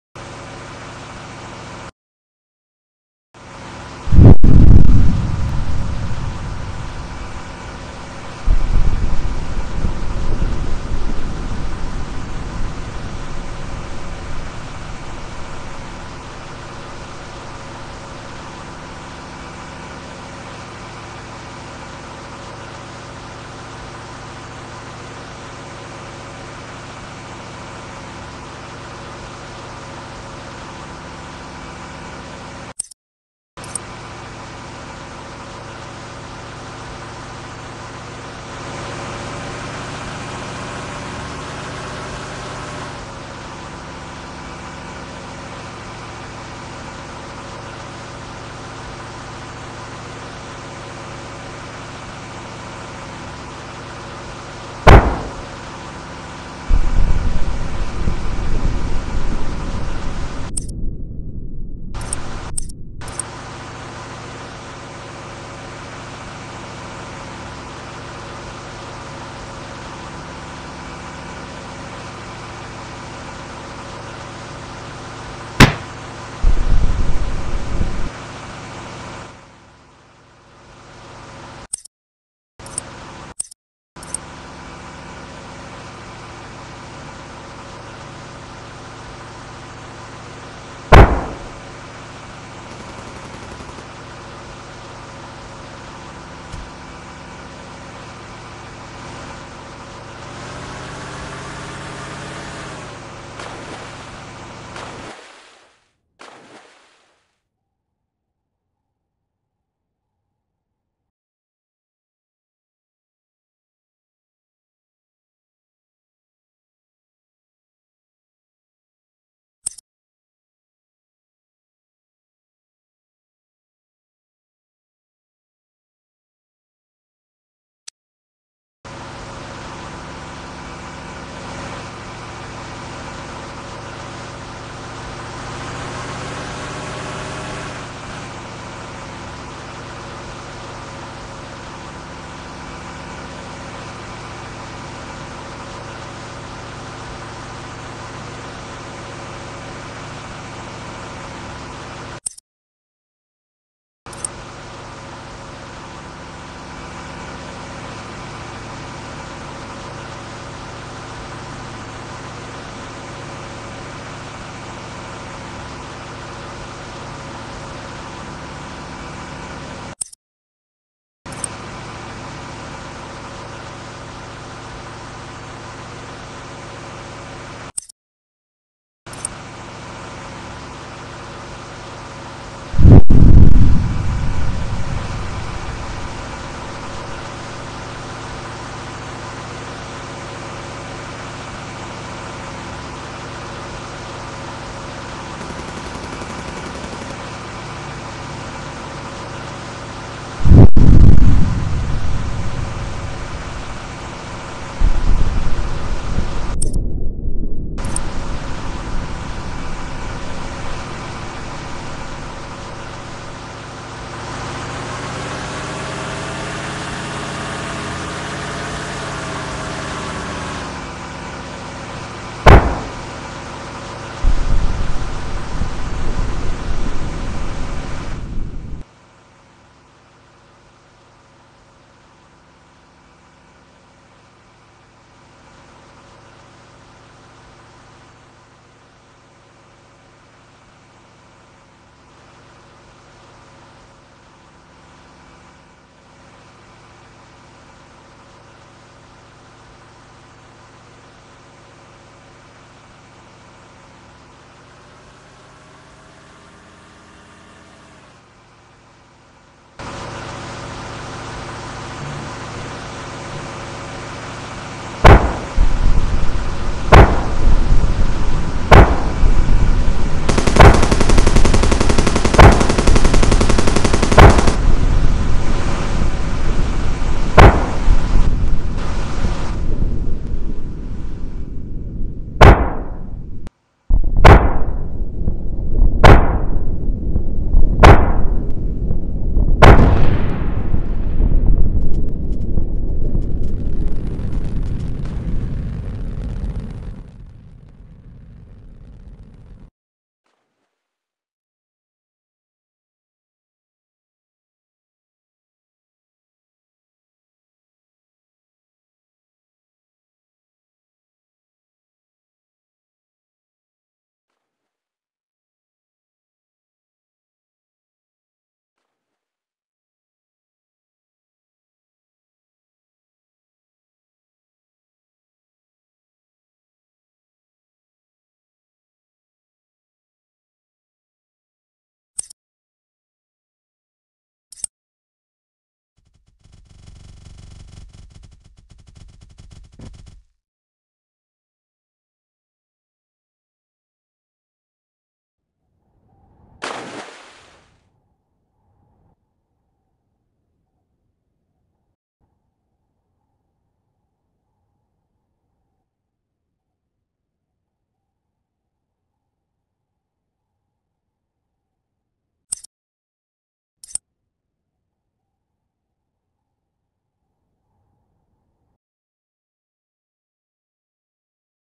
The Golden Tank Rampage
The Golden tank began to rampage through the Wright isles navy, is this a rogue project or a weapon of mass destruction?